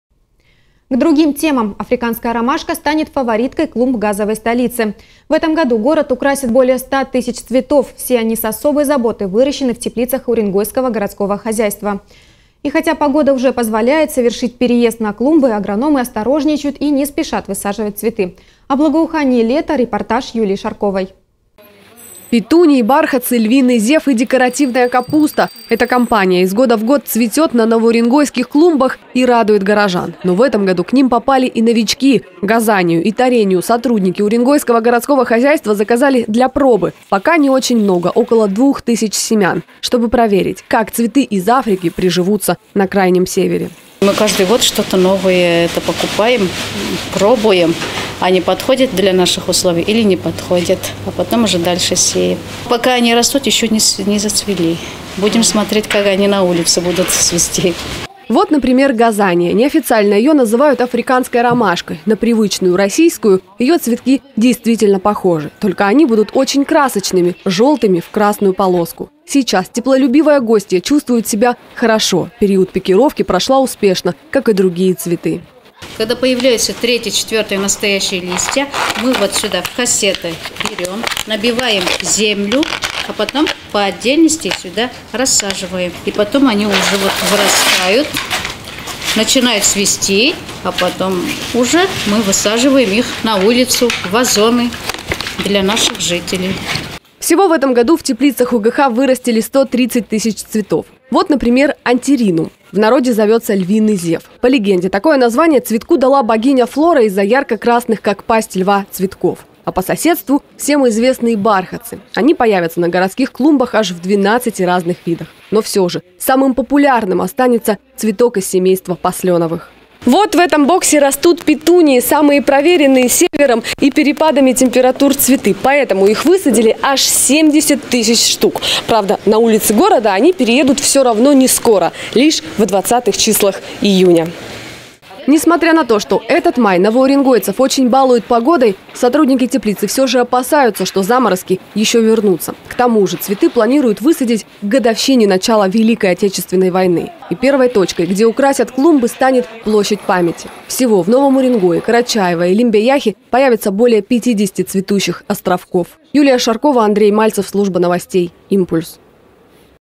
В Новом Уренгое вырастили более 130 тысяч цветов для украшения городских клумб
Африканская ромашка станет фавориткой клумб газовой столицы. В этом году город украсят более ста тысяч цветов. Все они с особой заботой выращены в теплицах «Уренгойского городского хозяйства». И хотя погода уже позволяет совершить «переезд» на клумбы, агрономы осторожничают и не спешат высаживать цветы.